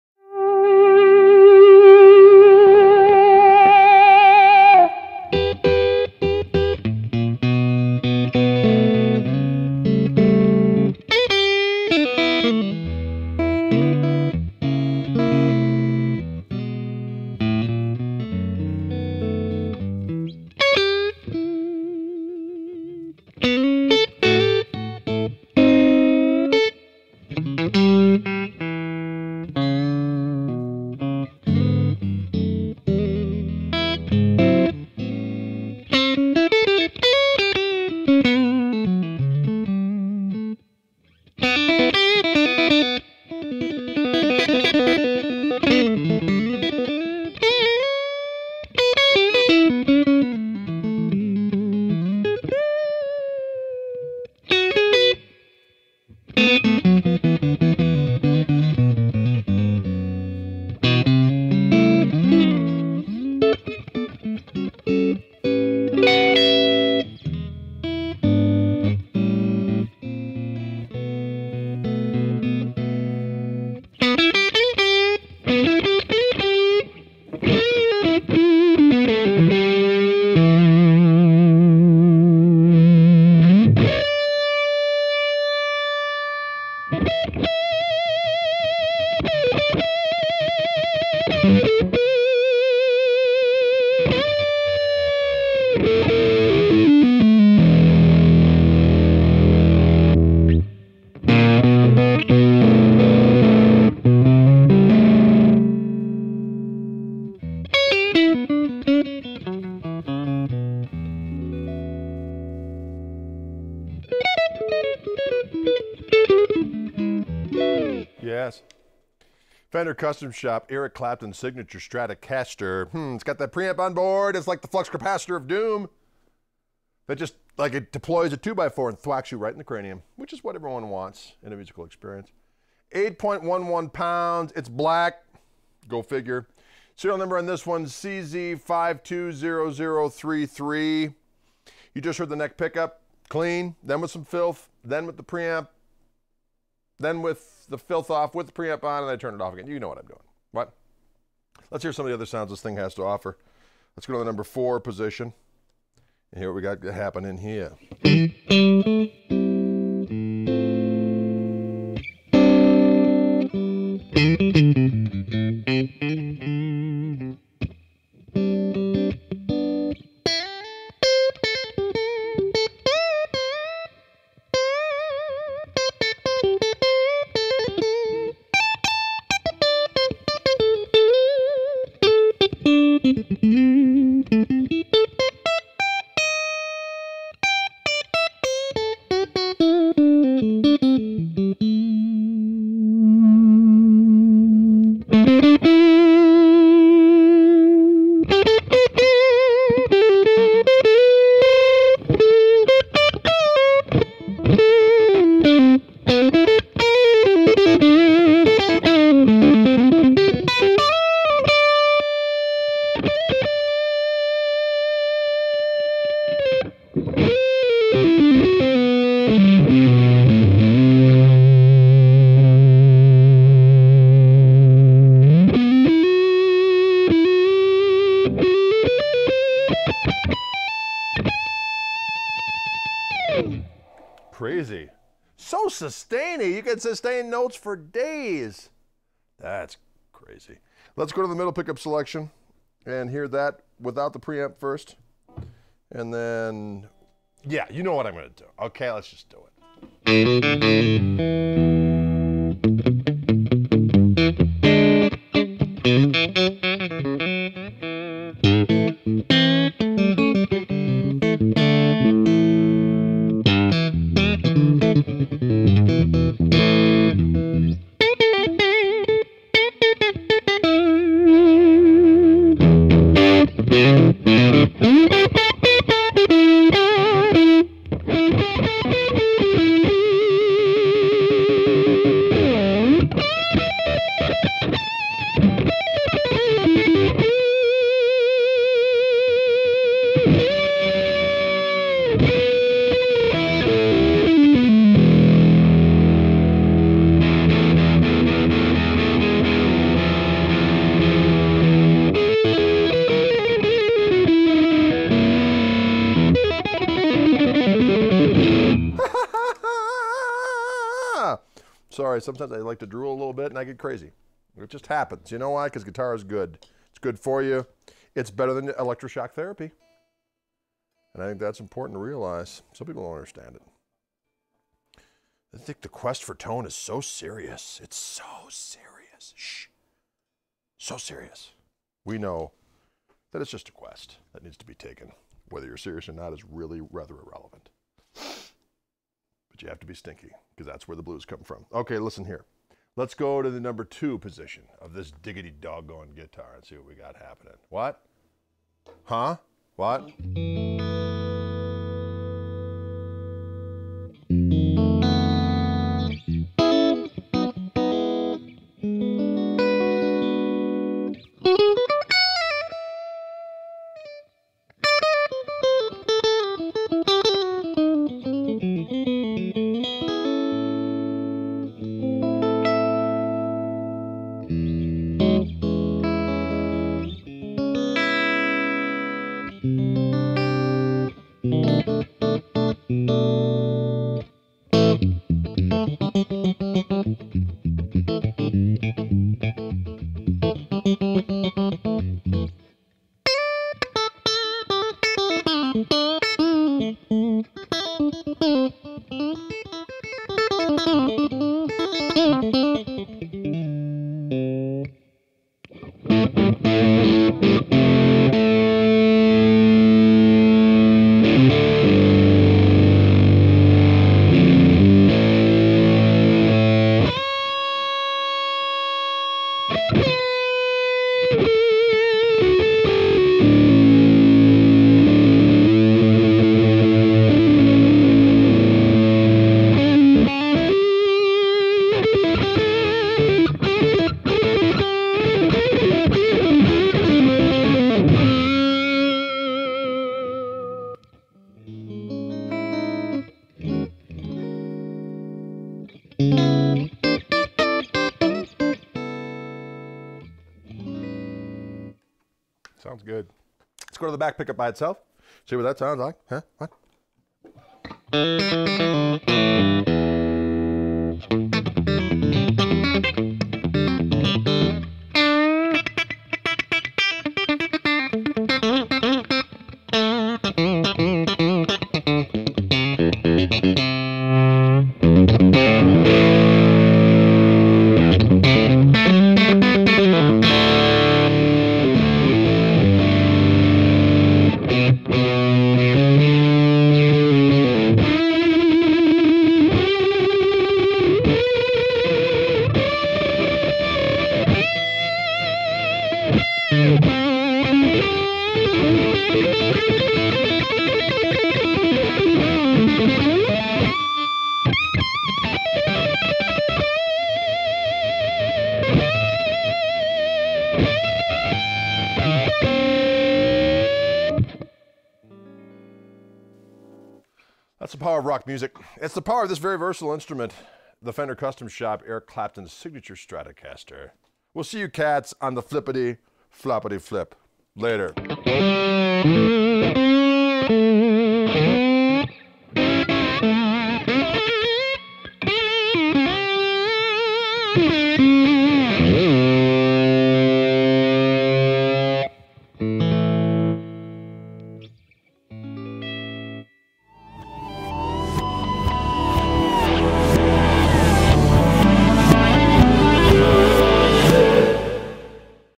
Fender Custom Shop Eric Clapton Signature Stratocaster • SN: CZ520033
The Fender Custom Artist Series Eric Clapton Stratocaster is an exact Custom Shop representation of Eric's personal axe. It's even made by the same craftsmen who made the actual Clapton guitar. Refinements include a special soft V neck shape, Noiseless pickups, blocked original vintage synchronized tremolo bridge, active mid-boost circuitry, and master TBX tone control. Standard features include an alder body, bone nut, vintage frets, chrome hardware, and Eric Clapton's signature on the headstock. Includes case.

Serial Number: CZ520033
Brand: Fender Custom Shop
Model: Eric Clapton Signature Stratocaster
Type: Stratocaster
Signature: Eric Clapton
Finish Color: Black
Weight: 8.11 Lbs
Body Wood: Select Alder
Neck Wood: 1-Piece Maple
Neck Shape: Soft "V"
Fingerboard: Maple
Fingerboard Radius: 9.5"
Scale Length: 25.5"
Width at Nut: 1.650"
Frets: 22 Vintage Style Frets
Pickups: 3 Vintage Noiseless
Controls: Volume, Tone, Active Mid-boost, 5-Position Blade Switch
Hardware : Nickel/Chrome
Tailpiece: "Blocked" American Vintage Synchronized Tremolo
Tuners: Fender/Gotoh Vintage Style
Pickguard: 1-Ply White
Unique Features: Active Midrange Boost, Eric Clapton's Signature on Headstock
Case: Vintage Tweed Case
Case Candy: Strap and Cable